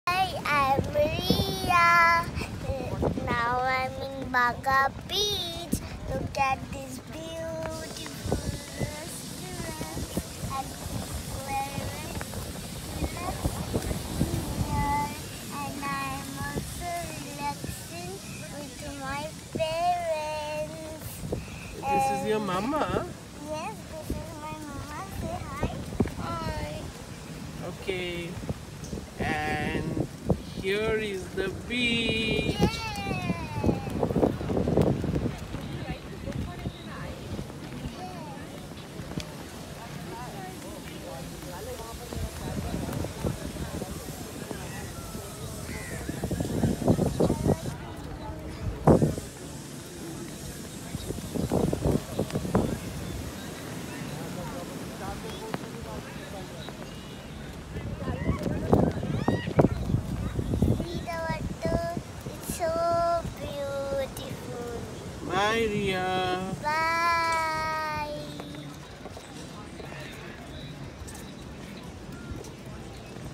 Baga Beach, Goa. Riya TV #1
Baga Beach is one of the most famous beaches of goa , It has some of the best shacks on the beach along with the famous Britto's.